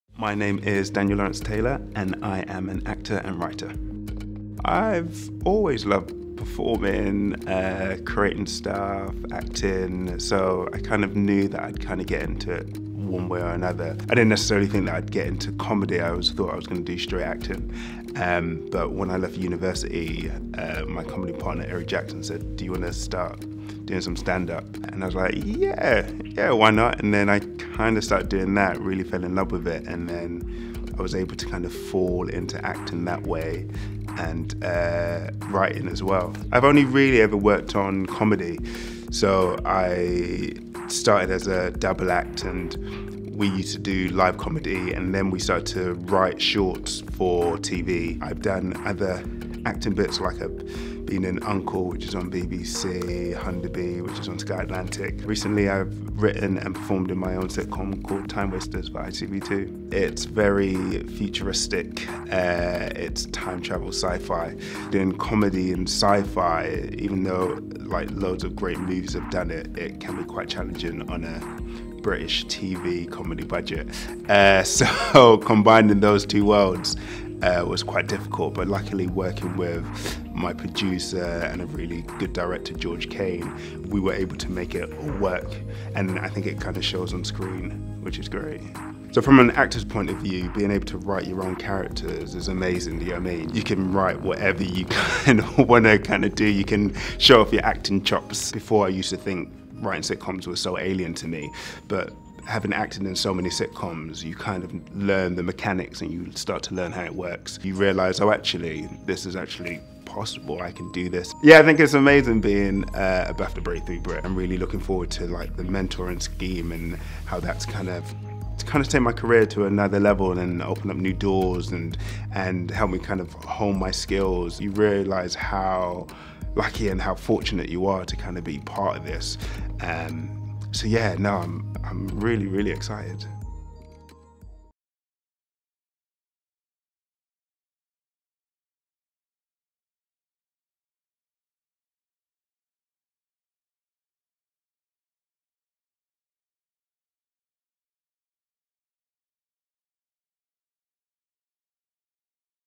Writer-Actor Daniel Lawrence Taylor | Breakthrough Brits 2018
Daniel always saw himself playing dramatic roles, but comedy came knocking at university, when he found himself winning laughs in comic plays and sketch shows. After a great experience at the Edinburgh Festival he formed double act Ginger and Black with Eri Jackson, and started writing more comedy, including BAFTA-nominated scripted comedy series, Timewasters. He credits his family with giving him his sense of humour, and remembers growing up with a lot of well-natured teasing. He has named many of his characters after his siblings and says he’s always cheeky about representing them on screen, but never horrible!

Daniel is glad to see television starting to take diversity seriously and hopes the industry remains committed to it. He is particularly proud to be writing the kind of black characters rarely seen on television and hopes to inspire and provide opportunities for black actors. He also hopes that Timewasters is a showcase for what’s possible, even without an Oxbridge leg-up.

Daniel wants to keep acting and creating, and would love to continue writing for television as well as breaking into film. He hugely admires Michaela Coel and jokes it as his intention to become a great friend of hers, whether she likes it or not.